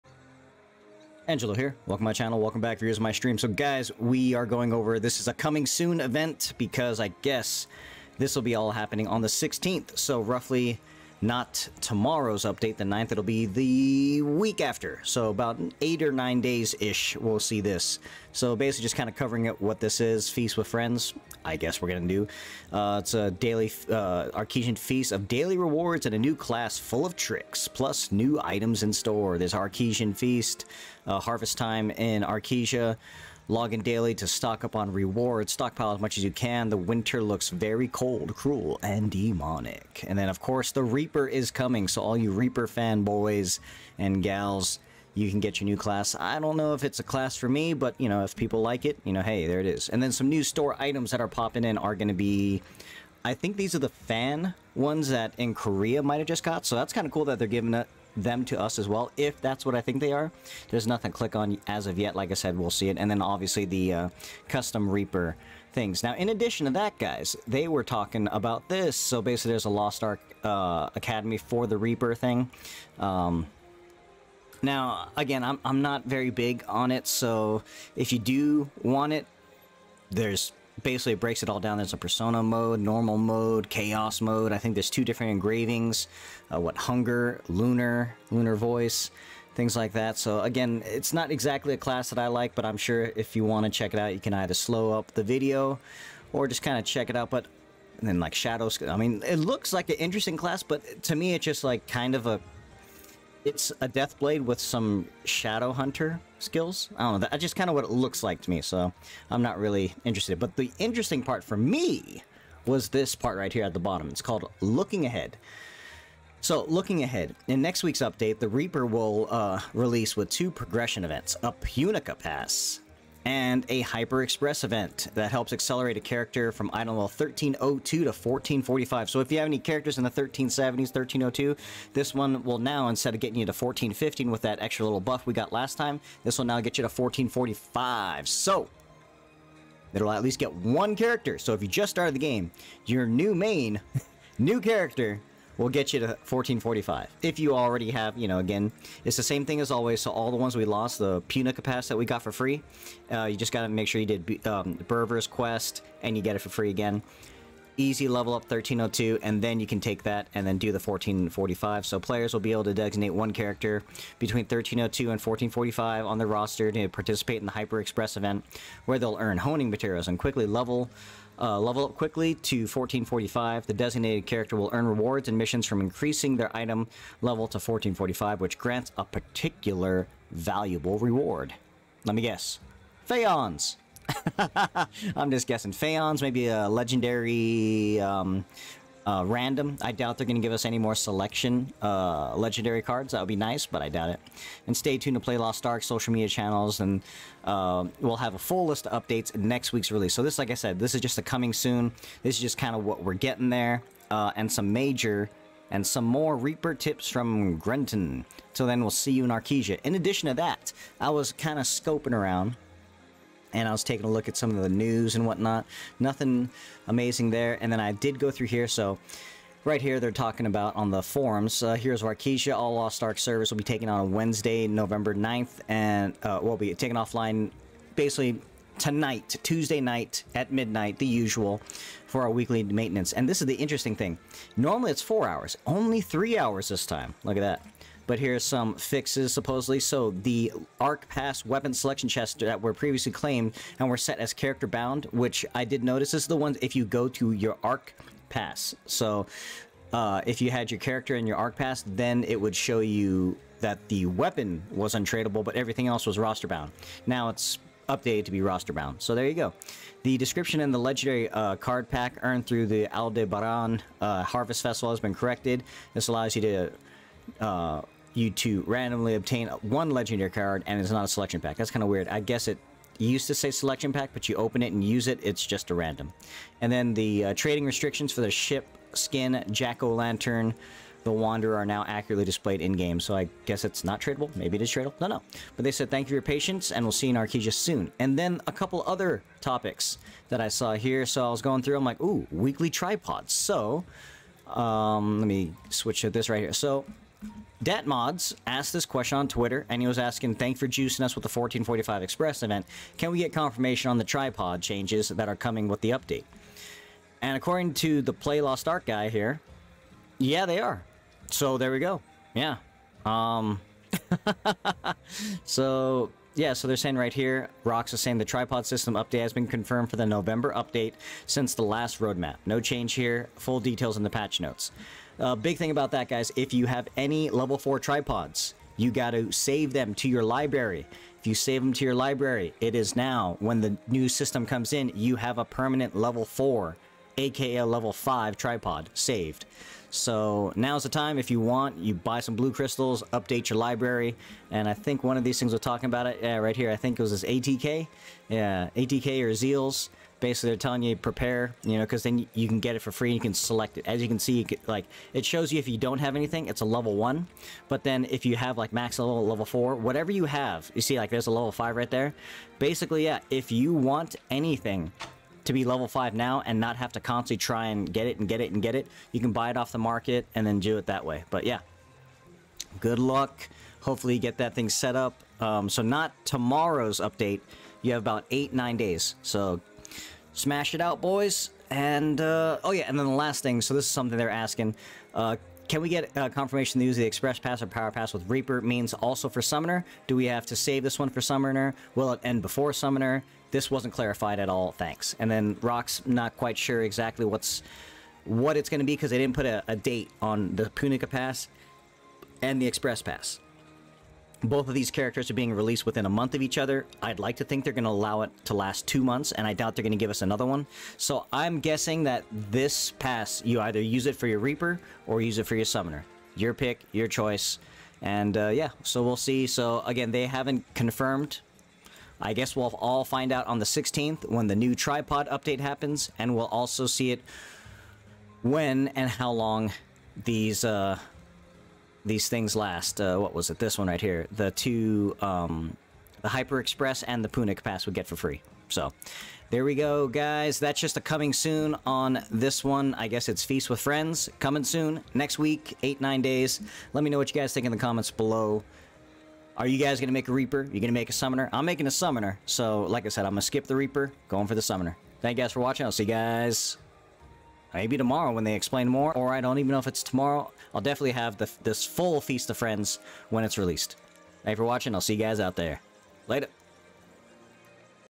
Breakdown of the "Coming Soon" Feast with Friends Update and Some Forum Questions on Powerpasses!
An GeL LoW here breaking down the "Coming Soon" Feast with Friends Update!

I also check out what other news came out on playlostark.com and the community forums!

Thanks as always for watching and I will see you out there!  Later!